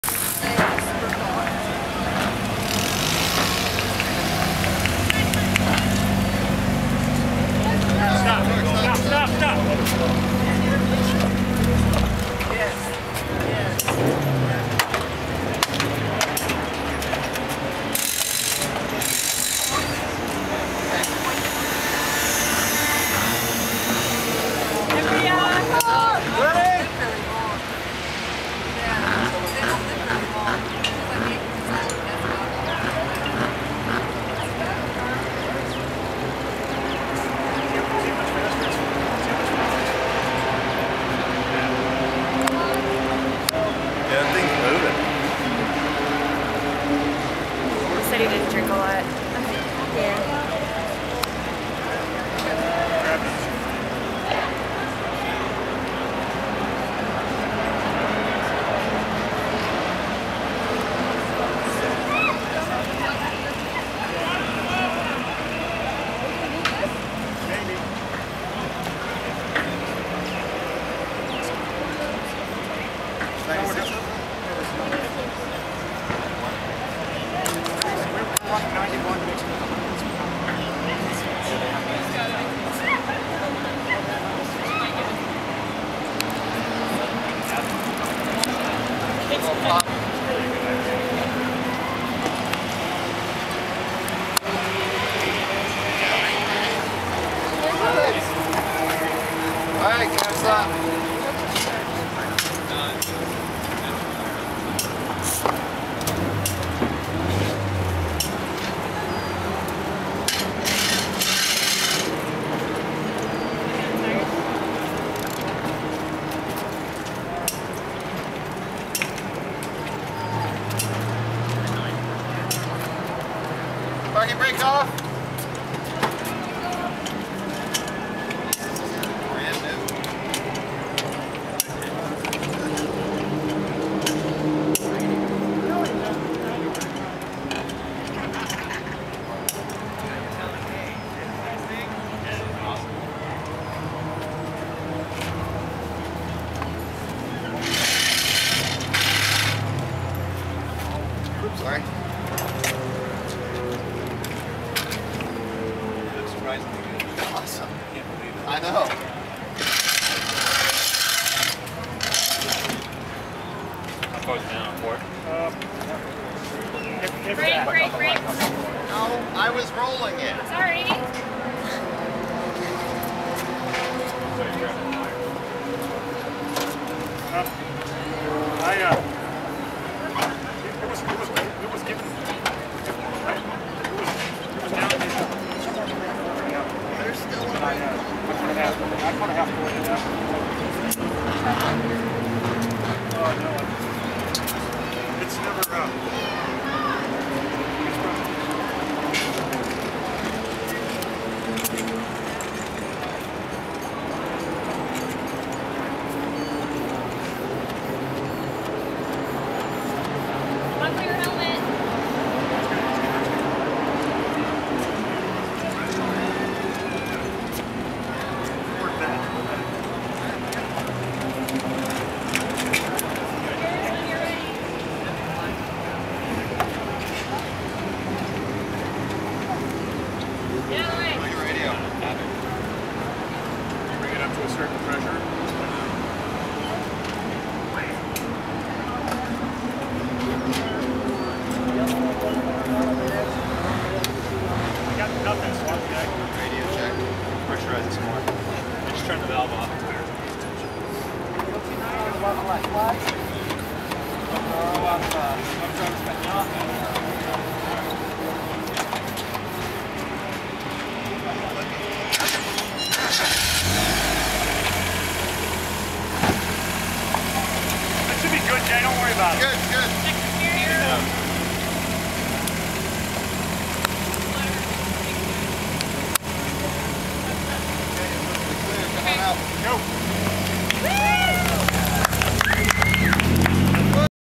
Eyesore Racing Pit Stop at Sears Pointless
One of our longer pits at 4+ minutes. Apologies for dirty, scratched camera lens.